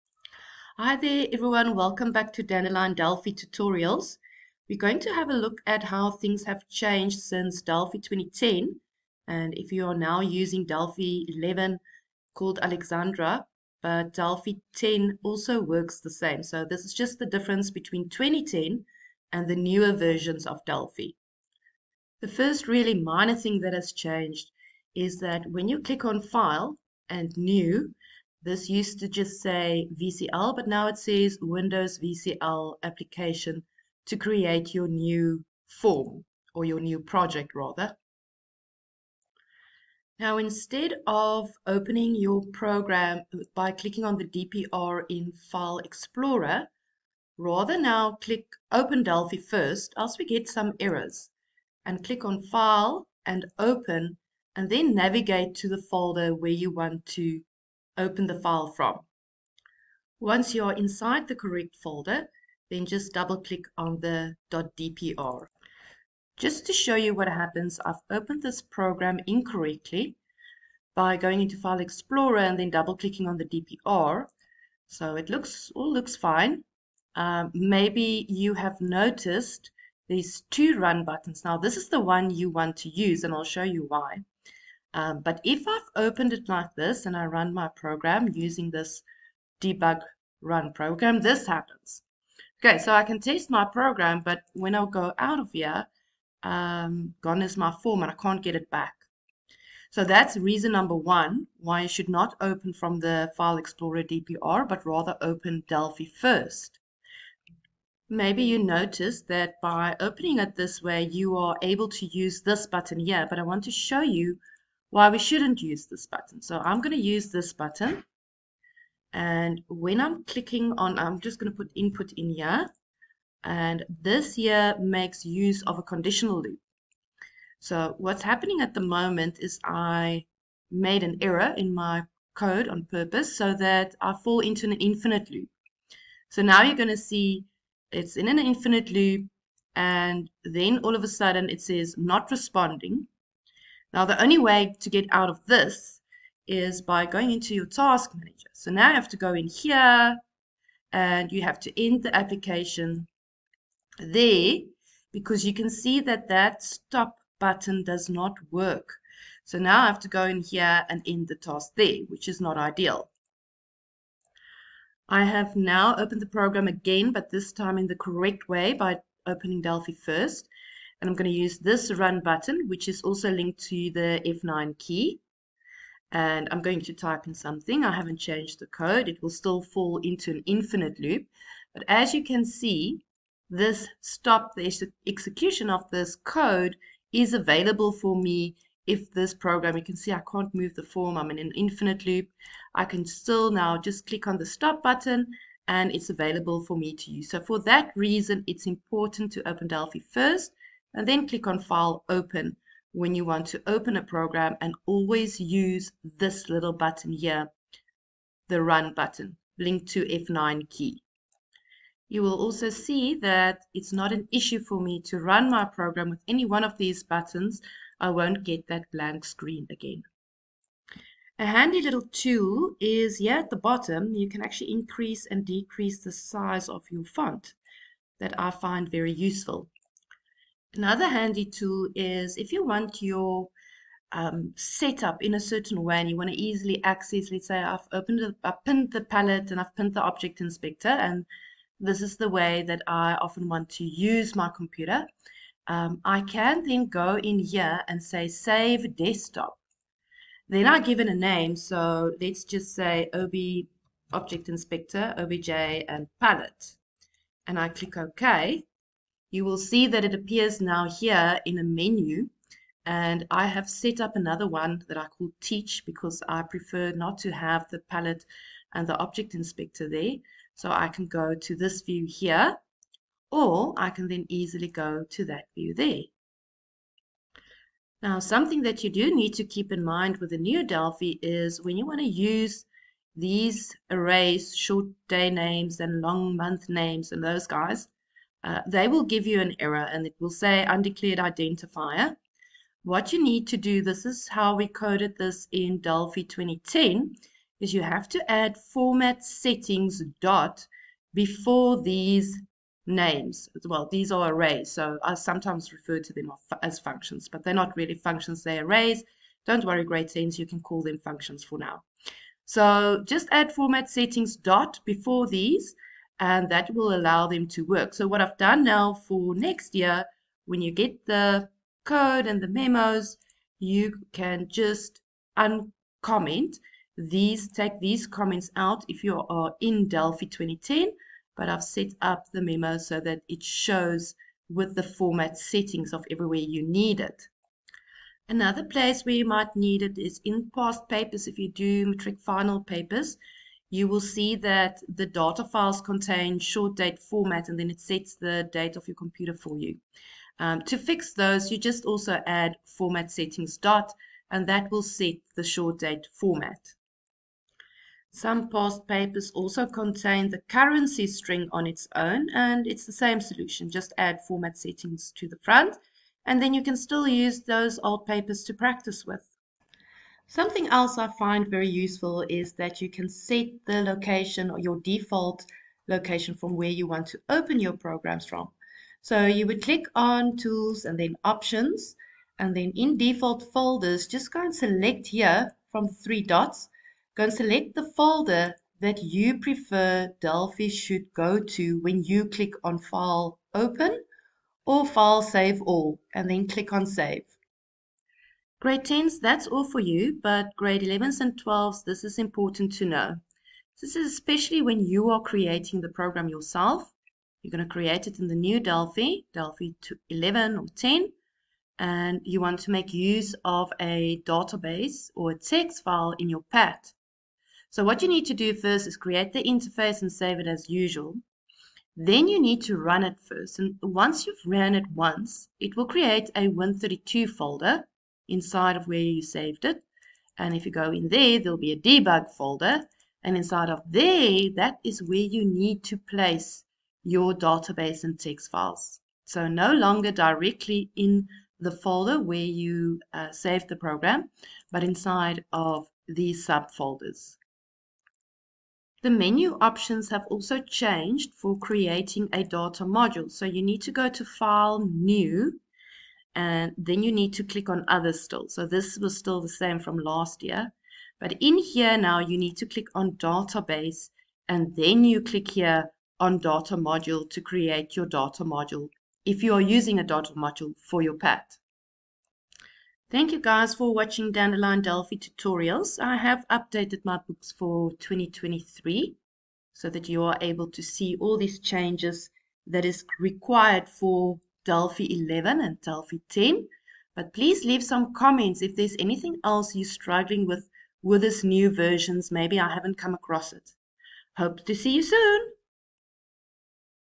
Changes from Delphi 2010 to Delphi 10 and 11
A few settings and changes to code to take note of that changed from using Delphi 2010 to the newer version, Delphi 10 and 11.